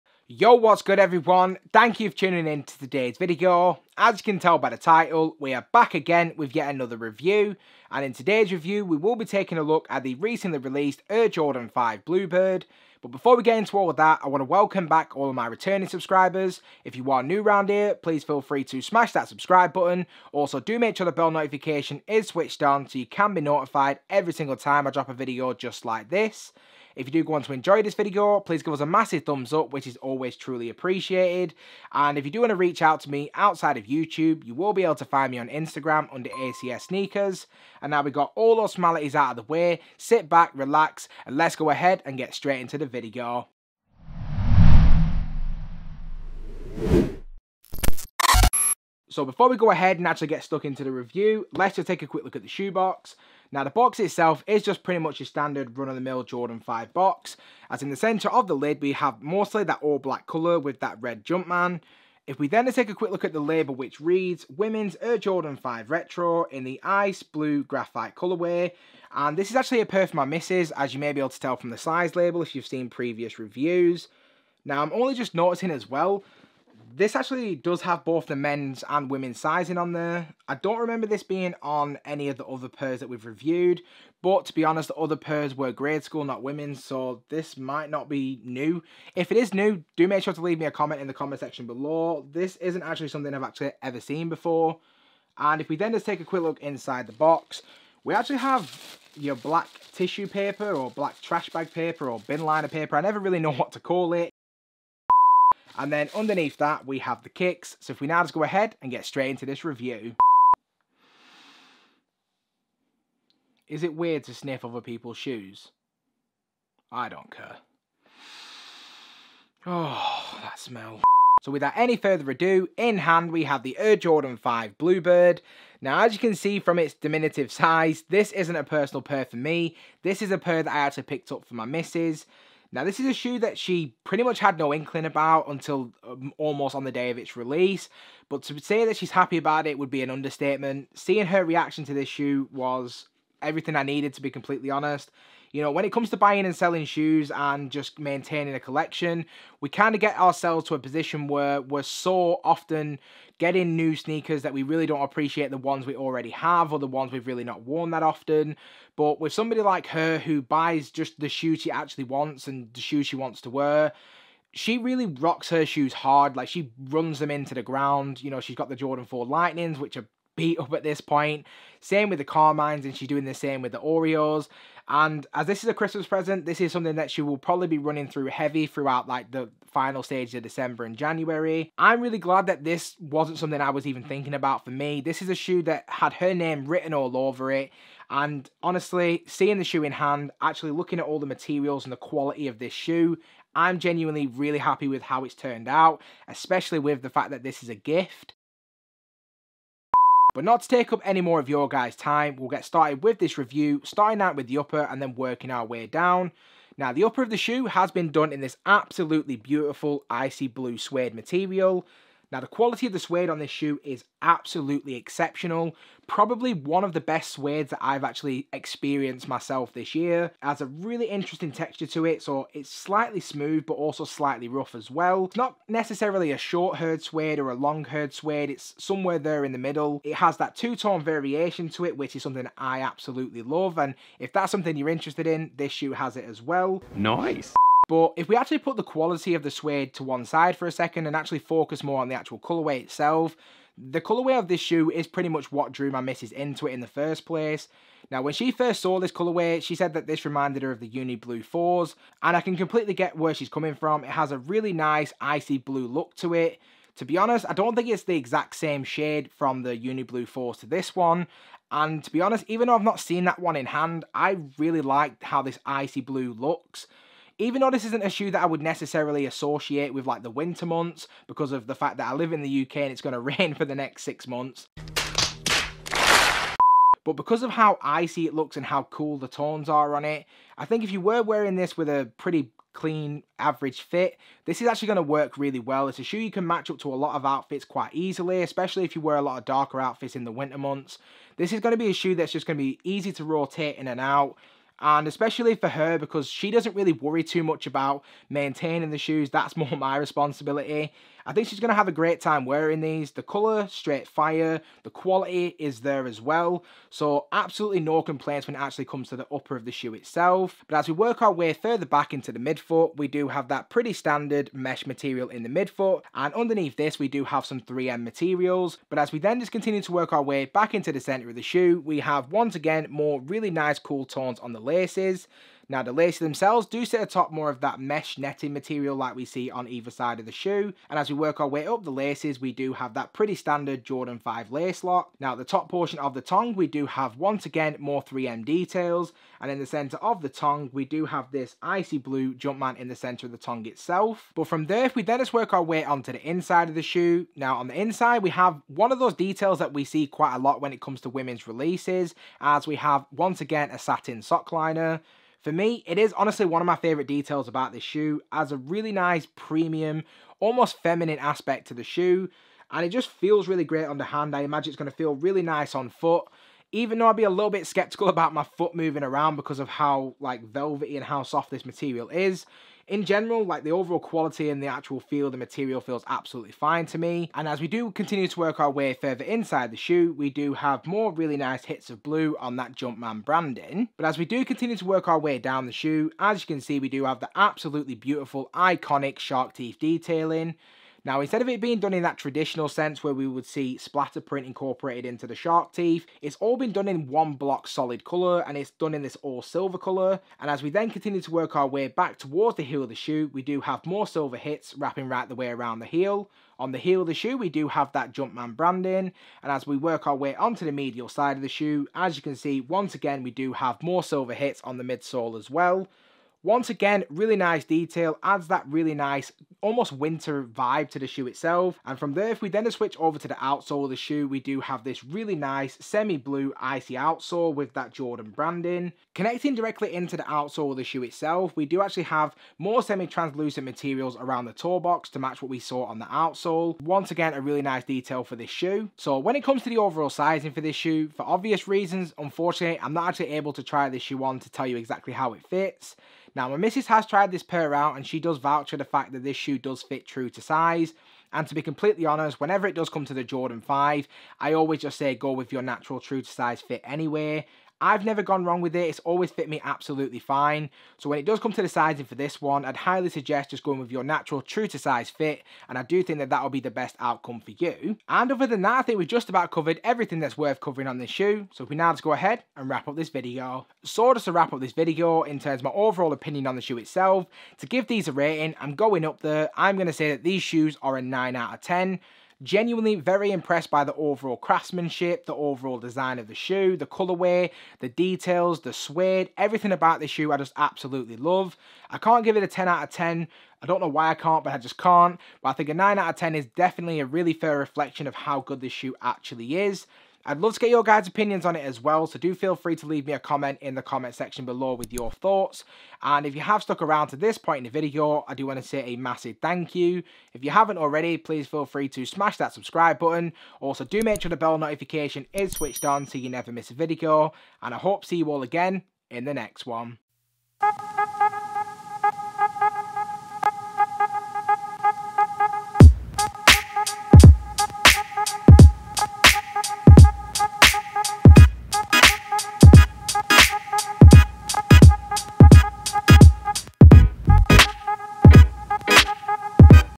SO COLD!🥶 Air Jordan 5 'Blue Bird' Detailed Review
Welcome back to ACS Sneakers, today we are taking a look at the recently released women's exclusive Air Jordan 5 'Blue Bird'